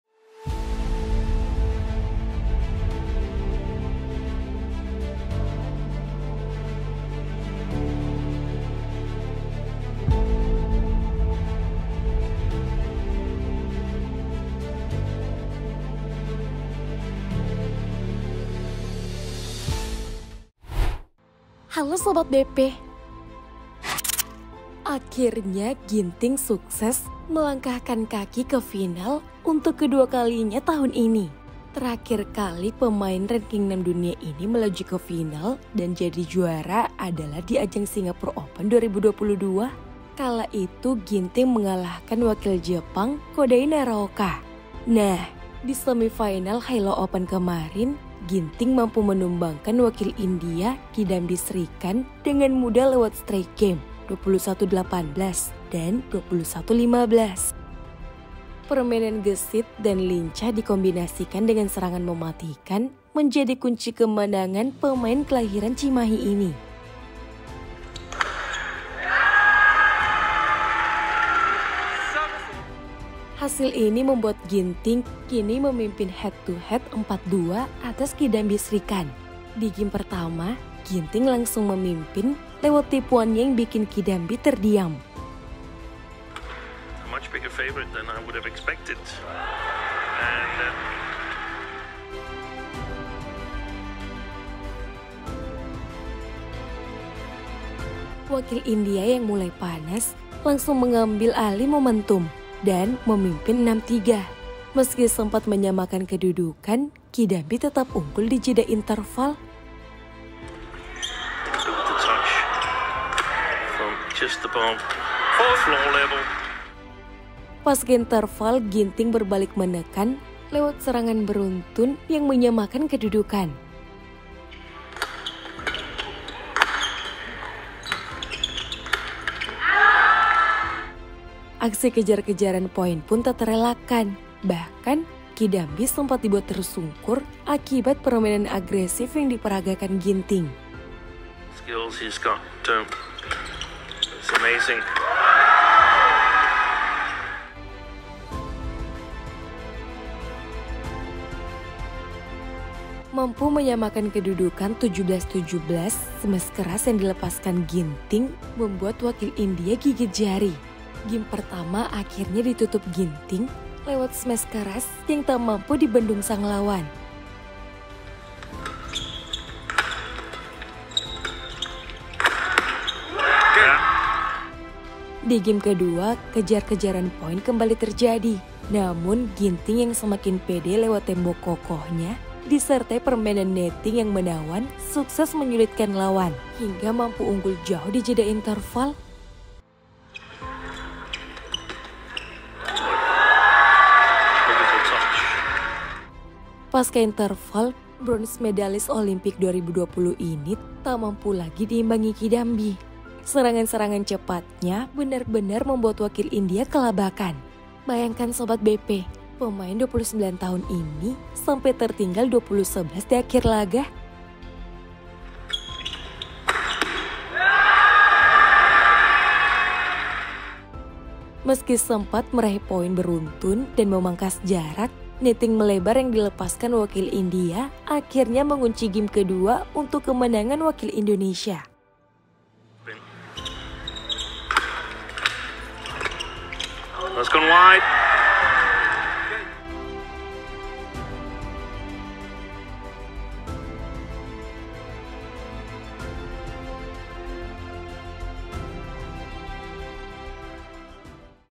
Udah Pake Ritual Masih Aja Kalah.!! Parade Smash Ginting Kalahkan Kidambi Srikanth di Hylo Open 2022
Akhirnya Ginting sukses melangkahkan kaki ke final untuk kedua kalinya tahun ini. Terakhir kali pemain ranking 6 dunia ini melaju ke final dan jadi juara adalah di ajang Singapore Open 2022. Kala itu Ginting mengalahkan wakil Jepang Kodai Naraoka. Nah, di semifinal Hylo Open kemarin, Ginting mampu menumbangkan wakil India Kidambi Srikanth dengan mudah lewat straight game 21-18 dan 21-15. Permainan gesit dan lincah dikombinasikan dengan serangan mematikan menjadi kunci kemenangan pemain kelahiran Cimahi ini. Well, Hasil ini membuat Ginting kini memimpin head to head 4-2 atas Kidambi Srikanth.